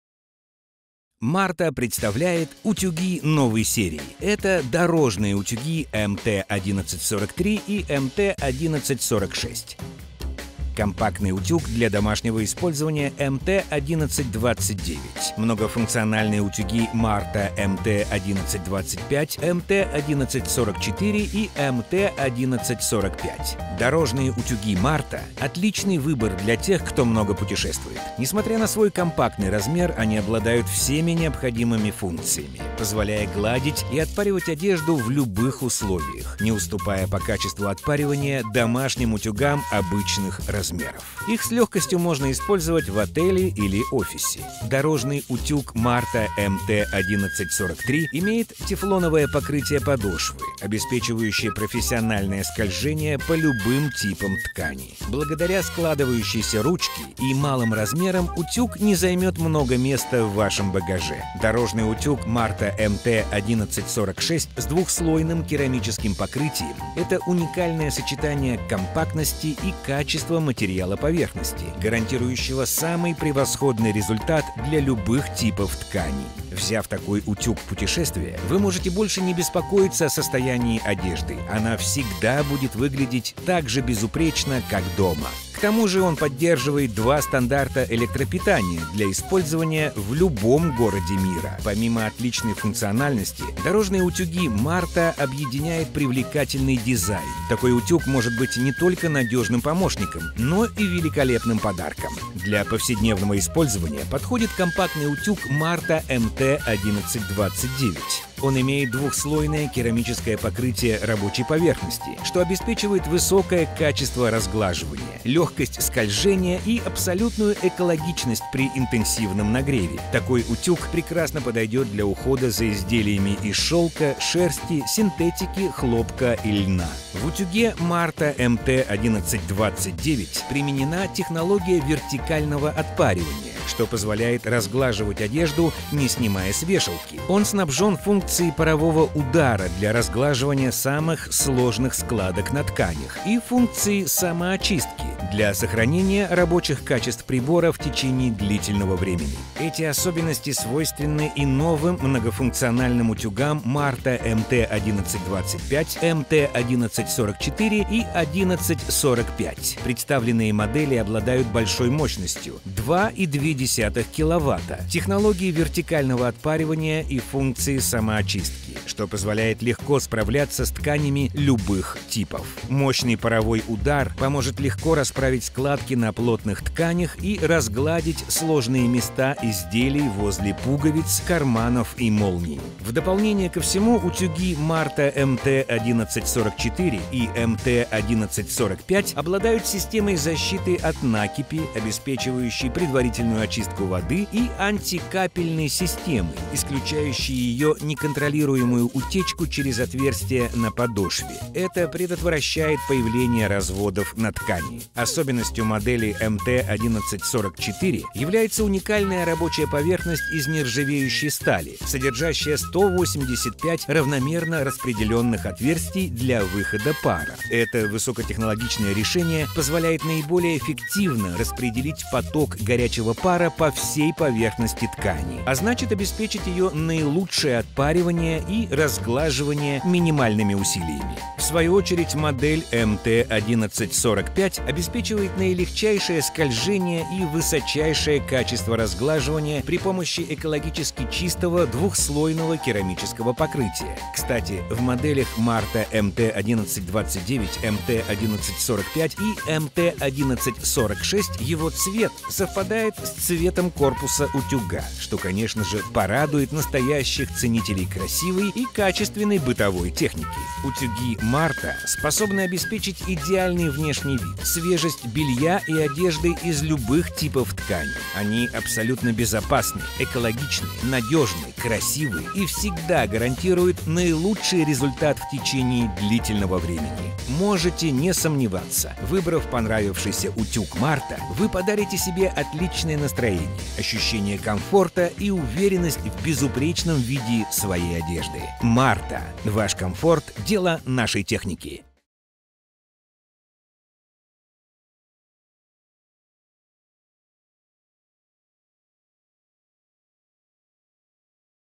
multimarta Обзор дорожных утюгов | Компактные и многофункциональные утюги MARTA
MARTA представляет утюги новой серии.

Это дорожные утюги MT-1143 и MT-1146,
компактный утюг для домашнего использования MT-1129,
многофункциональные утюги MARTA MT-1125, MT-1144 и MT-1145.

Дорожные утюги MARTA – отличный выбор для тех, кто много путешествует. Помимо отличной функциональности дорожные утюги MARTA объединяет привлекательный дизайн – такой утюг может быть не только надежным помощником, но и великолепным подарком.

Для повседневного использования подходит компактный утюг MARTA MT-1129. В нем применена технология вертикального отпаривания, что позволяет разглаживать одежду, не снимая с вешалки. Он снабжен функцией парового удара для разглаживания самых сложных складок на тканях, и функцией самоочистки - для сохранения рабочих качеств прибора в течение длительного времени.

Эти особенности свойственны и новым многофункциональным утюгам MARTA MT-1125, MT-1144 и 1145. К тому же они обладают системой защиты от накипи, обеспечивающей предварительную очистку воды, и антикапельной системой, исключающей ее неконтролируемую утечку через отверстия на подошве. Это предотвращает появление разводов на ткани.

Особенностью модели MT-1144 является уникальная рабочая поверхность из нержавеющей стали, содержащая 185 равномерно распределенных отверстий для выхода пара.

Утюги MARTA способны обеспечить идеальный внешний вид, свежесть белья и одежды из любых типов тканей, они абсолютно безопасны, экологичны, надежны, красивы и всегда гарантируют наилучший результат в течение длительного времени.

Можете не сомневаться - выбрав понравившийся утюг MARTA, Вы подарите себе отличное настроение, ощущение комфорта и уверенность в безупречном виде своей одежды.

MARTA. Ваш комфорт – дело  нашей техники!

0:00 MARTA представляет дорожные утюги
0:13 MARTA представляет компактный утюг MT-1129
0:17 MARTA представляет многофункциональные утюги
0:24 Дорожные утюги MARTA
0:46 Тефлоновое покрытие подошвы
0:57 Складывающаяся ручка утюга
1:04 Керамическое покрытие подошвы
1:27 Два стандарта электропитания
1:36 Привлекательный дизайн
1:45 Утюг для повседневного использования
2:10 Вертикальное отпаривание
2:25 Самоочистка
2:31 Новые многофункциональные утюги
3:05 Защита от накипи и антикапельная система
3:30 185 отверстий